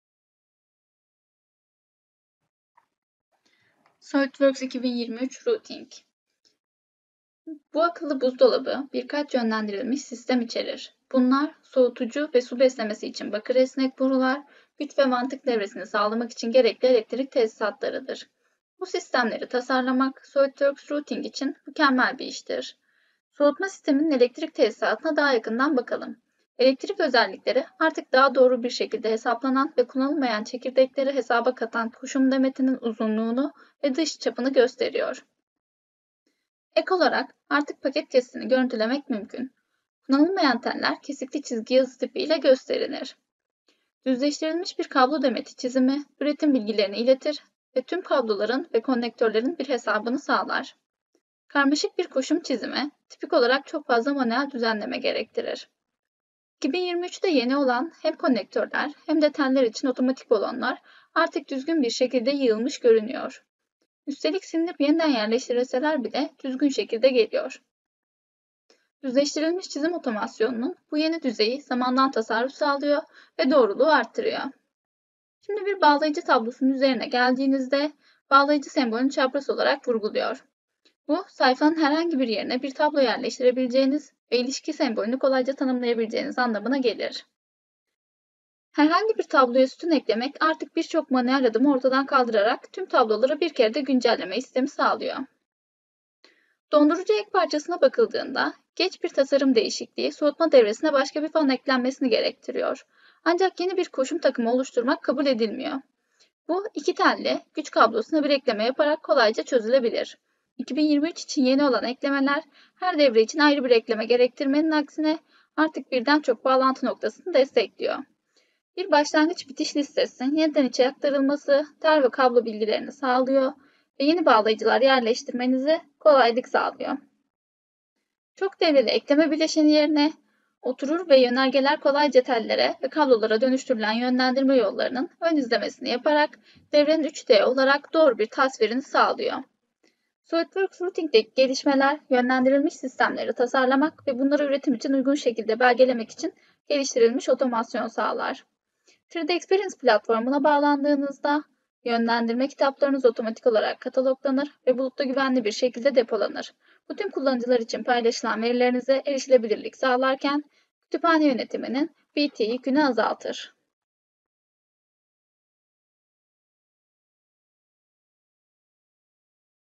SOLIDWORKS 2023 Yenilikleri | SOLIDWORKS Routing
Merakla beklenen SOLIDWORKS 2023 yenilikleri yayınlandı! SOLIDWORKS Routing'de 2023 ile birlikte gelen yenilikleri sizler için derledik.